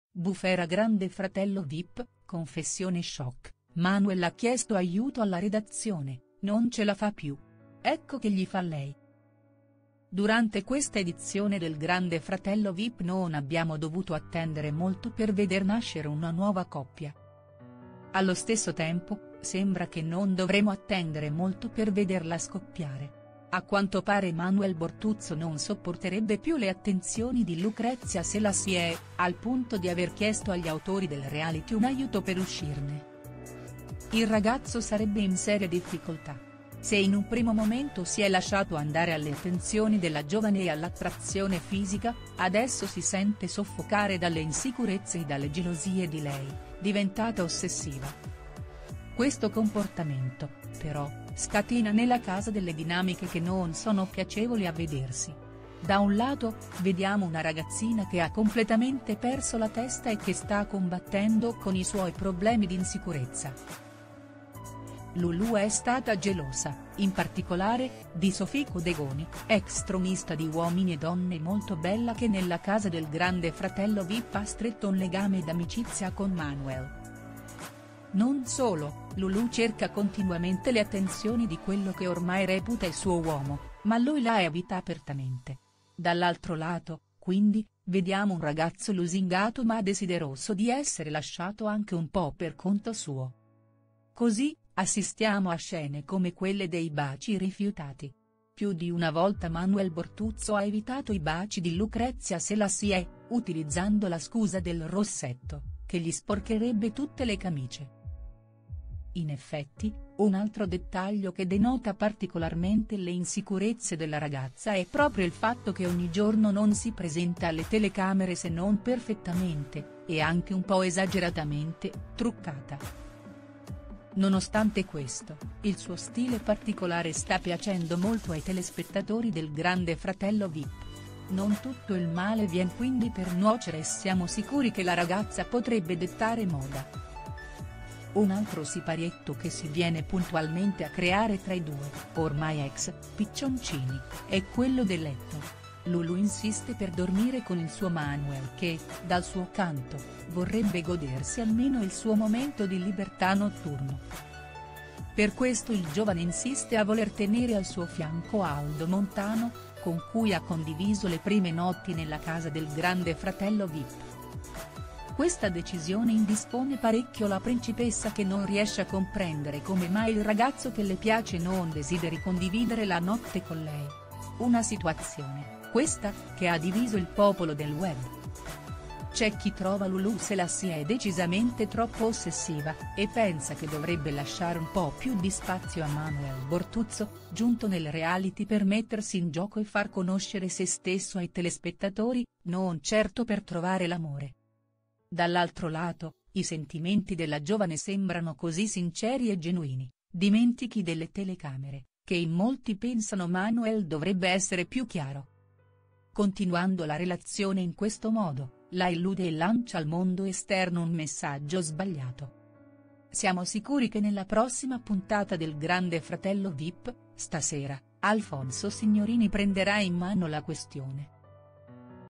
Bu.fera Grande Fratello Vip, Confessione ch.oc: “Manuel ha chiesto aiuto alla redazione, non ce...
Bu.fera Grande Fratello Vip, Confessione ch.oc: “Manuel ha chiesto aiuto alla redazione, non ce la fa più. Ecco che gli fa lei”